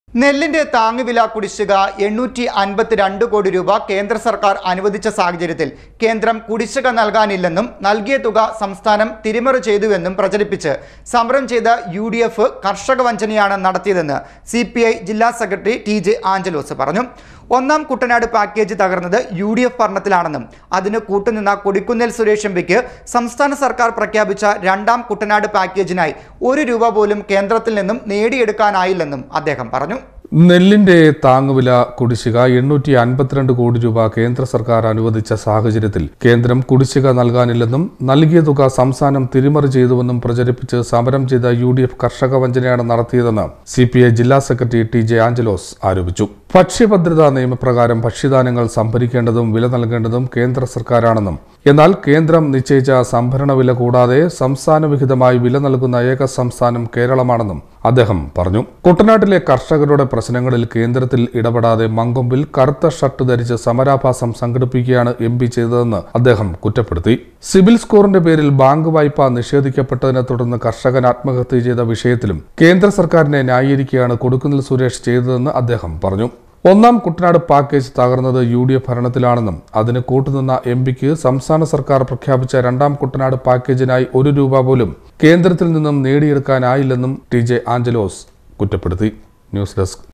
കര്‍ഷക വഞ്ചനയാണ് നടത്തിയതെന്ന് സി.പി.ഐ. ജില്ലാ സെക്രട്ടറി ടി.ജെ.ആഞ്ചലോസ്
നെല്ലിന്റെ താങ്ങുവില കുടിശിഖ 852  കോടി രൂപ കേന്ദ്ര സര്‍ക്കാര്‍ അനുവദിച്ച സാഹചര്യത്തില്‍ കേന്ദ്രം കുടിശിഖ നല്‍കാനില്ലെന്നും നല്‍കിയ തുക സംസ്ഥാനം തിരിമറി ചെയ്തുവെന്നും പ്രചരിപ്പിച്ച് സമരം ചെയ്ത  യു.ഡി.എഫ്  കര്‍ഷക വഞ്ചനയാണ് നടത്തിയതെന്ന് സി.പി.ഐ. ജില്ലാ സെക്രട്ടറി ടി.ജെ.ആഞ്ചലോസ്